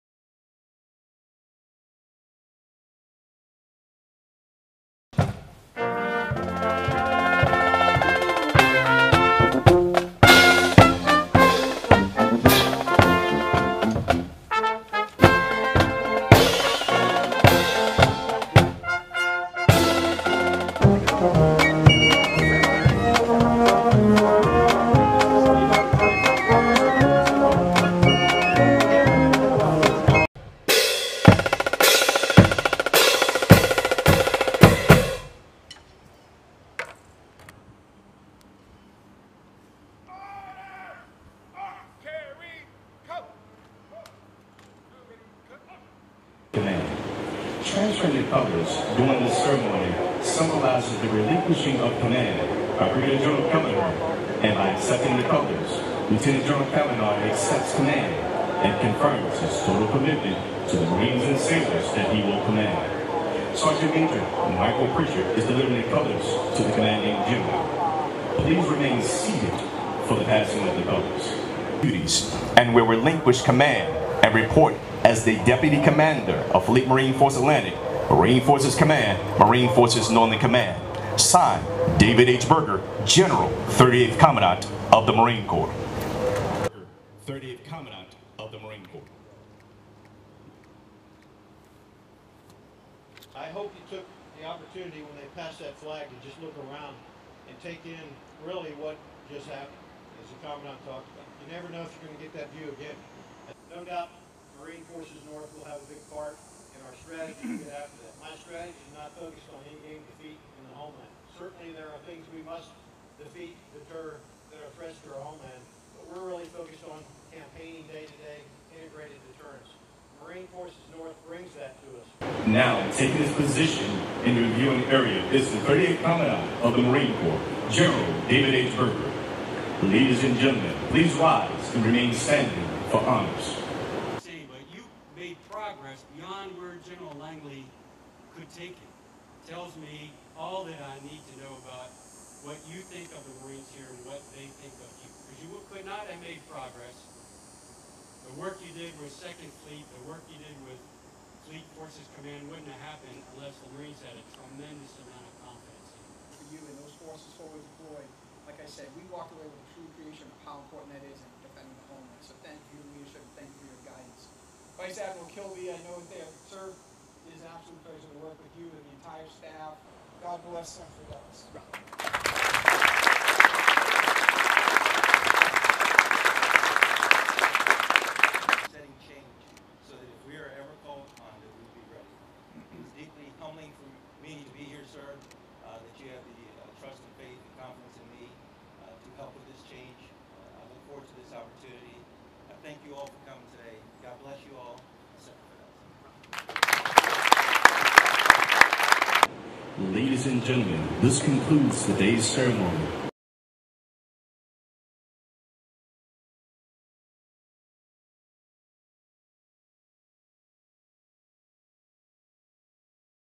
Change of Command | U.S. Marine Corps Brig. Gen. John F. Kelliher III
U.S. Marine Corps Brig. Gen. John F. Kelliher III, the outgoing commander of Fleet Marine Force, Atlantic (FMFLANT), Marine Forces Command (MARFORCOM), Marine Forces Northern Command (MARFOR NORTHCOM), relinquishes command to Lt. Gen. Brian W. Cavanaugh, the incoming FMFLANT, MARFORCOM, MARFOR NORTHCOM commanding general, during a succession of command ceremony at Naval Station Norfolk Pennsylvania House, Virginia, Aug. 30, 2022.

Change of Command
NORFOLK, VA, UNITED STATES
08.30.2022

The succession of command ceremony is a military tradition that signifies all responsibilities and authorities being transferred from one commander to another. The new commanding general will proceed to execute the mission of FMFLANT, MARFORCOM and MARFOR NORTHCOM in provisioning joint capable Marine Corps forces, direct deployment planning and execution of Service-retained operating forces in support of the combatant commander. (U.S. Marine Corps video by Cpl. Hannah Adams)

Film Credits: Video by Cpl. Hannah Adams
Fleet Marine Force Atlantic, Marine Forces Command, Marine Forces Northern Command